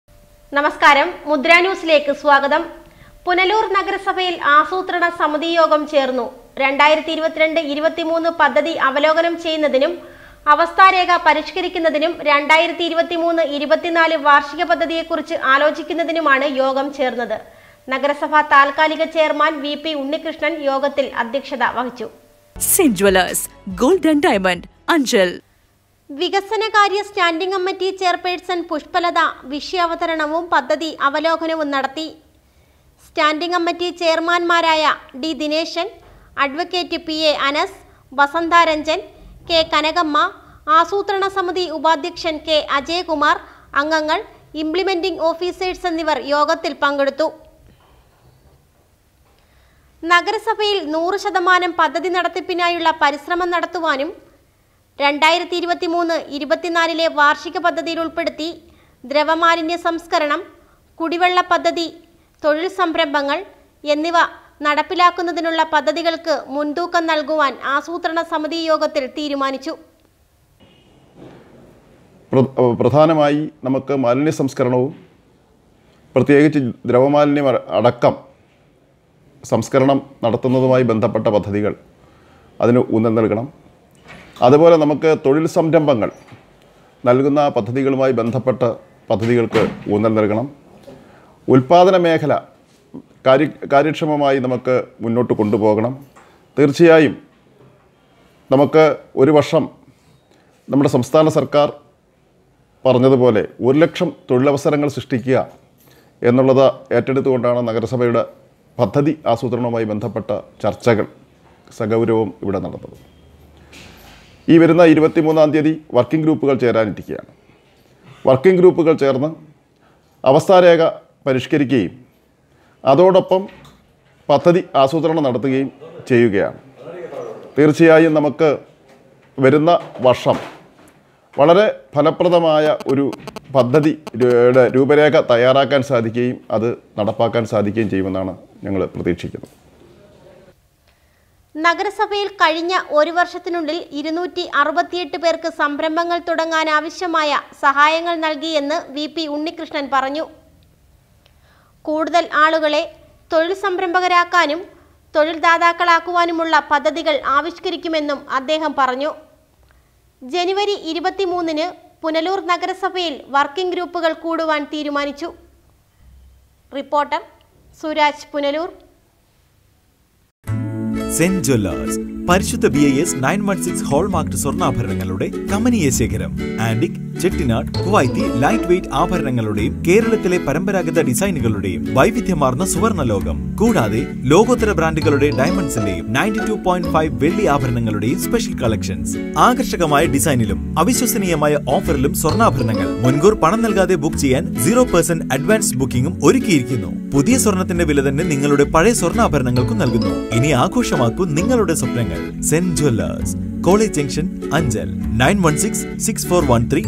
mudra,anchal news,mudra news,Punalur news,local news
പുനലൂർ നഗരസഭയിൽ ആസൂത്രണ സമതി യോഗം ചേർന്നു